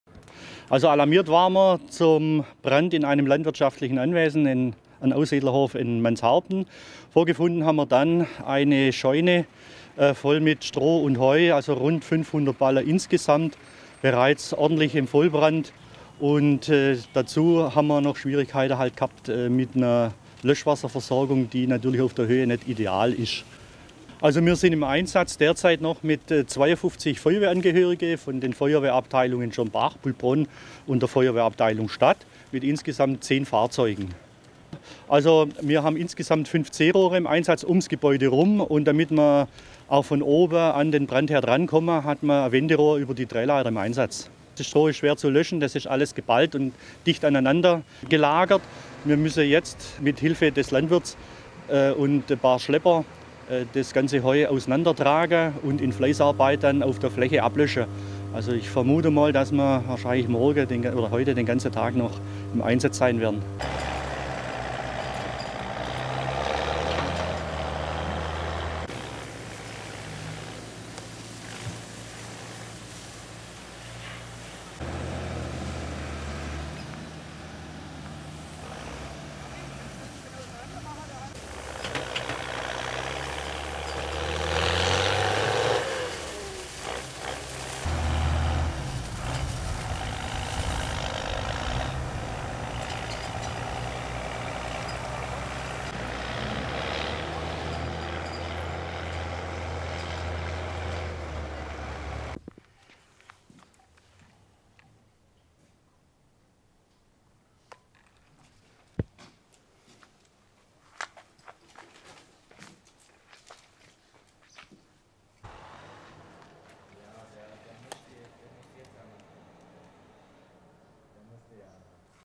26.08.2020 - Scheune mit über 800 Strohballen in Vollbrand
Schorndorf-Mannshaupten (pol) - Im Jägergarten kam es in der am Mittwochnacht gegen 23:20 Uhr zu einem  Brand in einer Scheune. In der Scheune waren etwa 800 Quaderballen Heu  und Stroh gelagert, welche Feuer fingen. Die Scheune geriet in  Vollbrand. Die Feuerwehren aus Schorndorf, Schornbach und Buhlbronn  waren mit 10 Fahrzeugen und 51 Feuerwehrleuten vor Ort und konnten eine  weitere Ausbreitung des Feuers verhindern. Durch den Brand entstand ein  Sachschaden von etwa 78 000 Euro. Es wurden keine Personen verletzt. Die  Löscharbeiten der Feuerwehr, sowie die Ermittlungen zur Brandursache  dauern an.